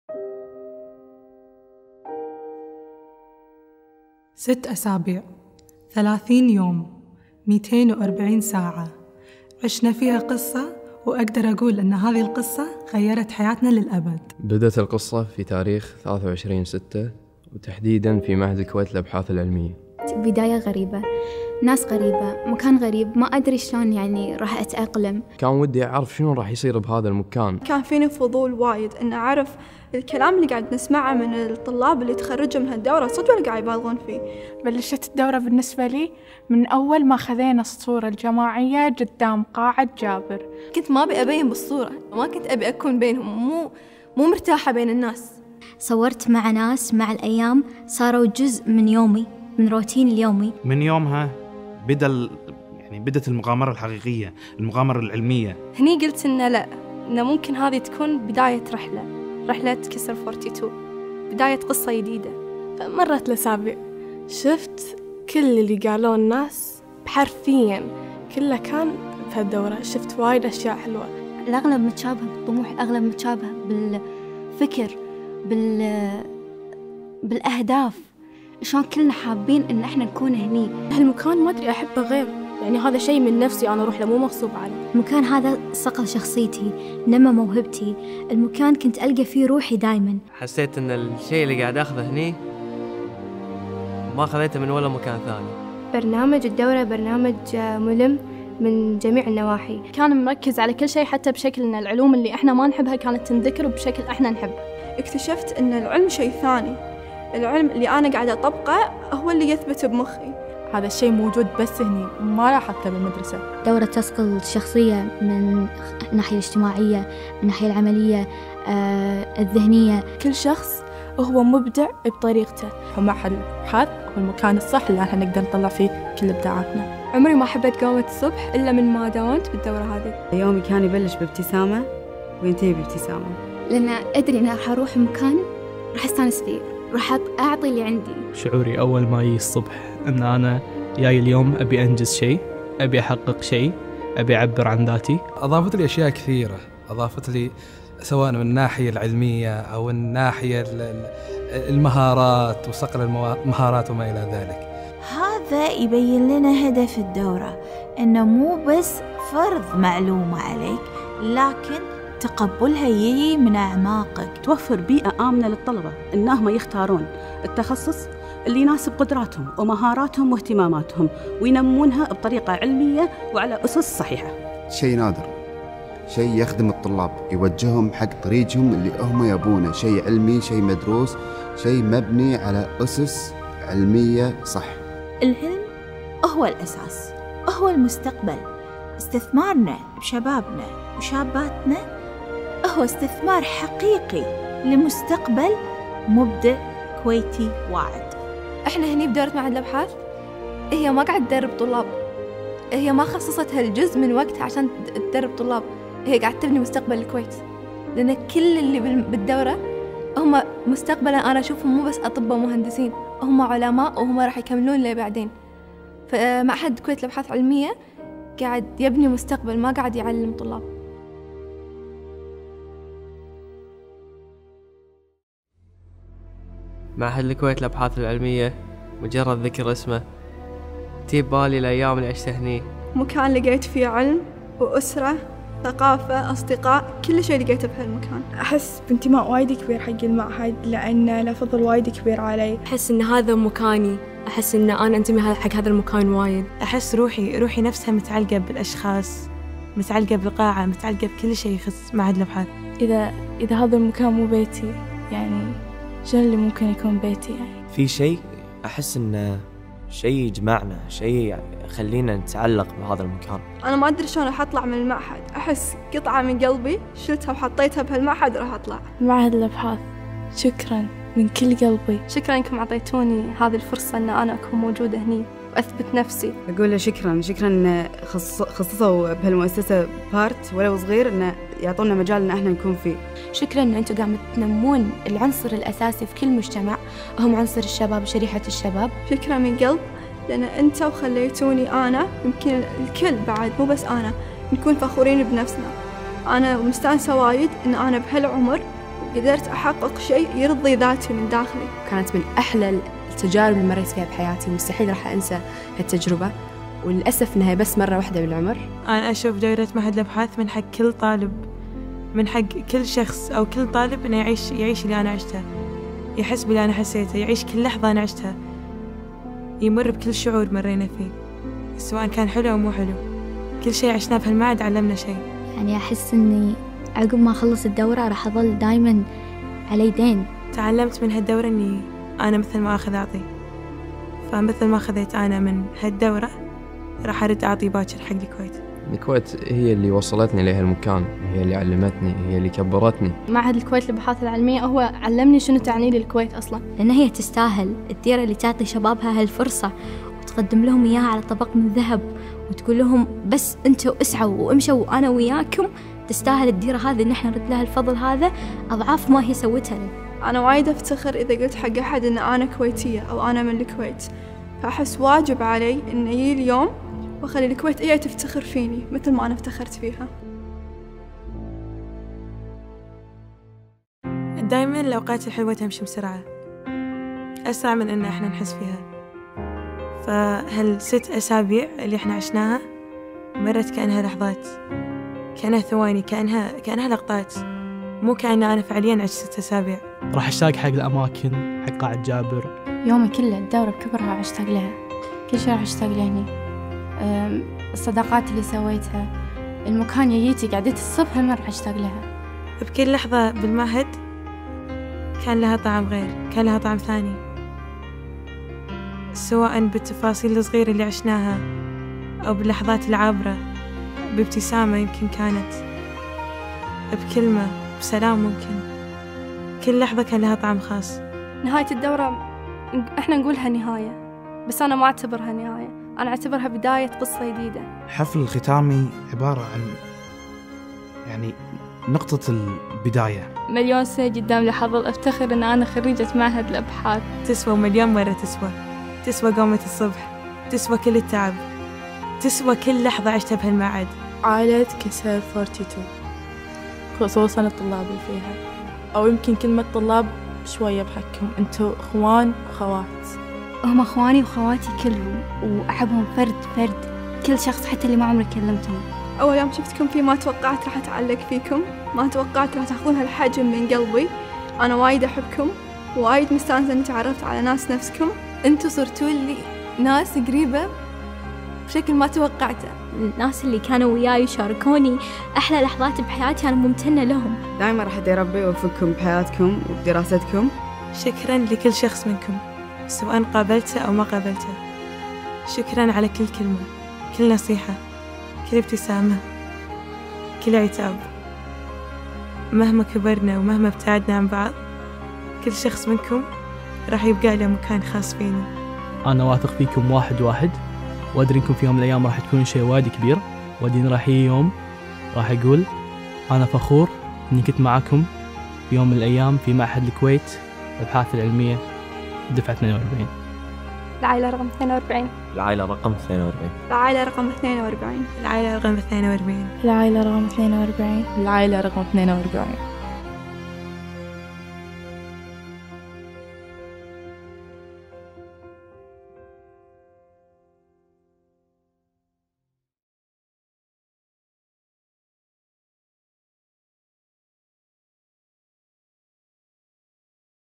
العائلة رقم ٤٢ | إهداء لجميع طلبة وطالبات الدورة التدريبية الـ٤٢ | KISR42
‎يصادف اليوم "يوم الشباب الدولي"، والذي أقرته الجمعية العامة للأمم المتحدة ١٢ أغسطس من كل عام

لذا يحرص المعهد دوما على تنظيم دورات ربيعية وصيفية تستهدف طلبة المرحلة المتوسطة، والثانوية، بالإضافة إلى طلبة الجامعات، تحقيقا لأحد أهدافه المتمثلة منذ نشأته في تشجيع الجيل الناشئ على ممارسة البحث العلمي، وإيمانا منه بأهمية إشراكهم في مختلف البرامج التنموية كونهم شركاء أساسيين في التغيير